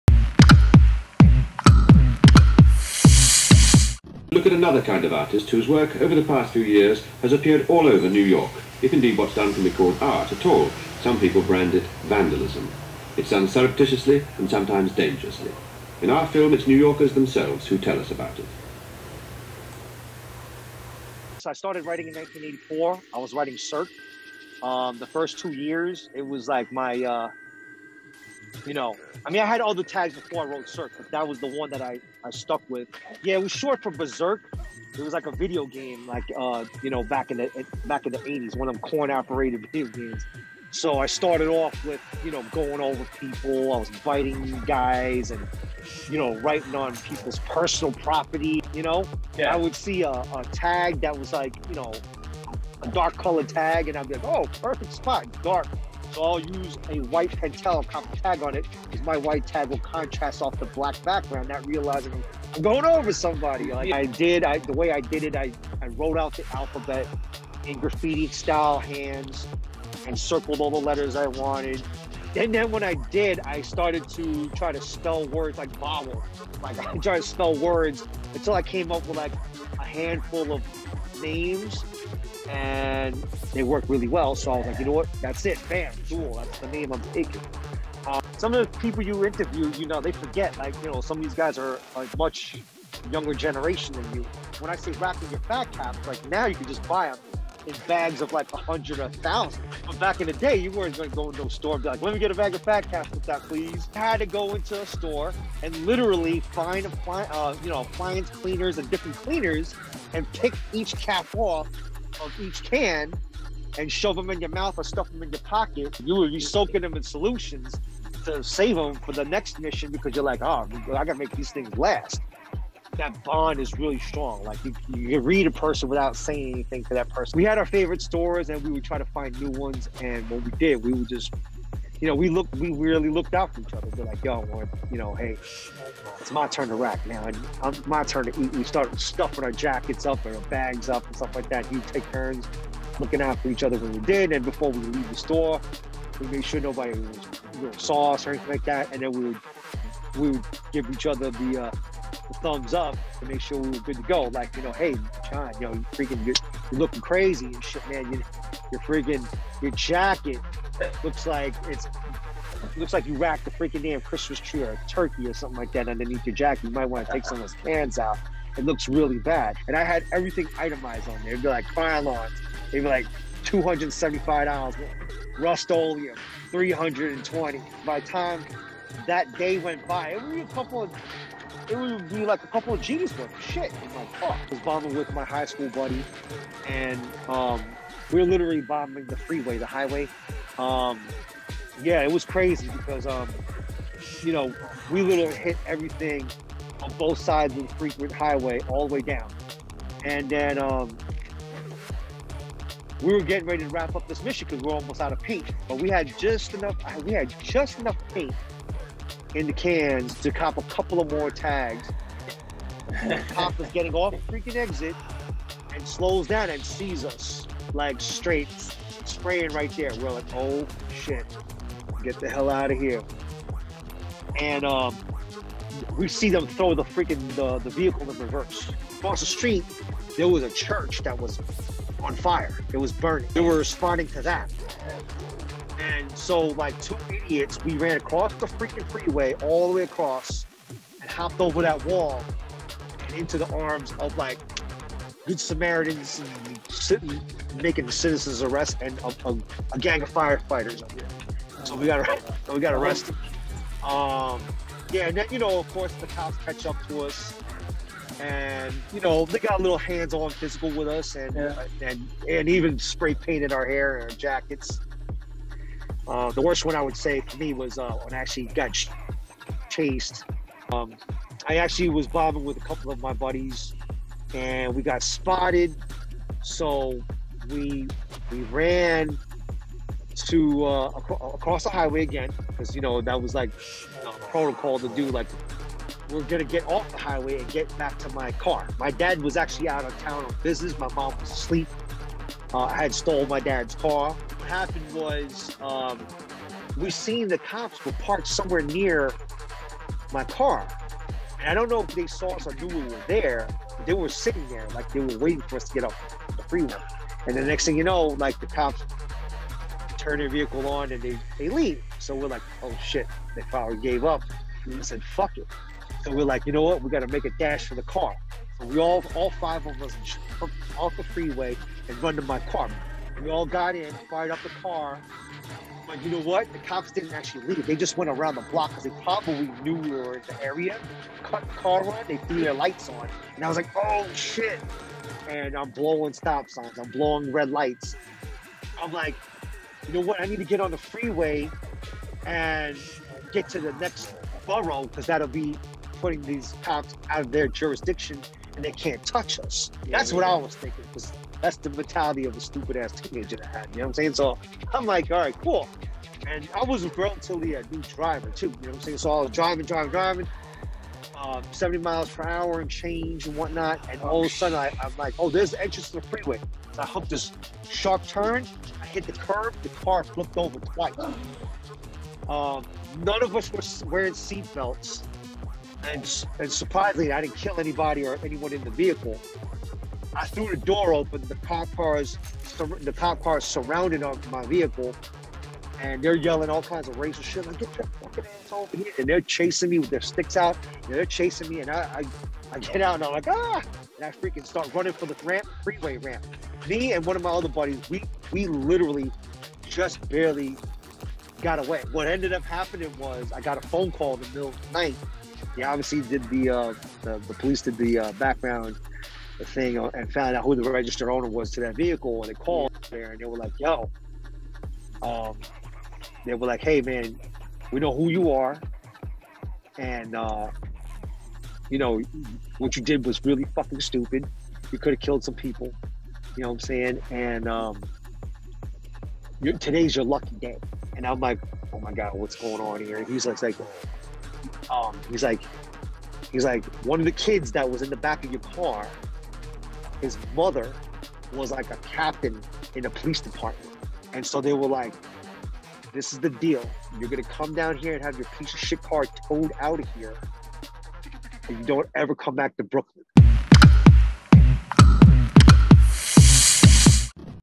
NEW YORK CITY GRAFFITI LEGEND DUEL: FROM THE GETTIN UP, TO THE RACKING, TO THE MISSON!
In todays exclusive mini Doc, we get into conversations with NYC legend and friend of the Kelavision channel, DUEL NYC.
We're talking early beginnings in NYC, Racking, Painting, the missions and more. This is a snapshot insight into the world of DUEL.